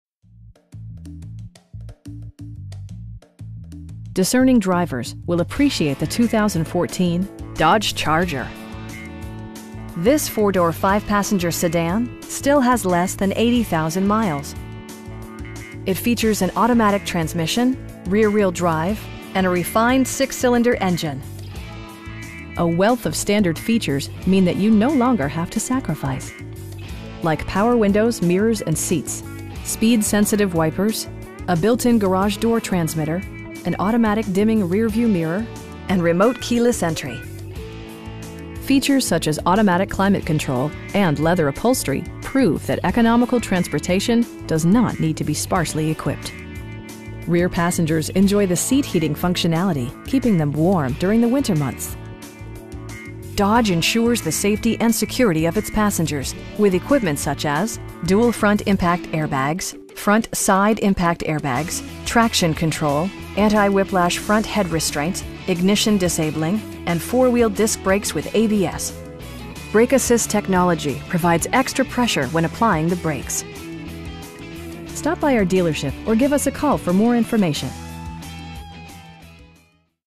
2014 Dodge Charger SXT in Moline, IL 61265
Sexton Ford
3802 16th Street

Discerning drivers will appreciate the 2014 Dodge Charger. This 4 door, 5 passenger sedan still has less than 80,000 miles! It features an automatic transmission, rear-wheel drive, and a refined 6 cylinder engine. A wealth of standard features means that you no longer have to sacrifice. Like power windows, mirrors, and seats, speed sensitive wipers, a built-in garage door transmitter, an automatic dimming rear-view mirror, front bucket seats, telescoping steering wheel, cruise control, and remote keyless entry. Features such as automatic climate control and leather upholstery prove that economical transportation does not need to be sparsely equipped. Rear passengers enjoy the seat heating functionality, keeping them warm during the winter months. Dodge ensures the safety and security of its passengers with equipment such as: dual front impact airbags, front side impact airbags, traction control, anti-whiplash front head restraints, ignition disabling, and 4 wheel disc brakes with ABS. Brake assist technology provides extra pressure when applying the brakes. We pride ourselves on providing excellent customer service. Stop by our dealership or give us a call for more information.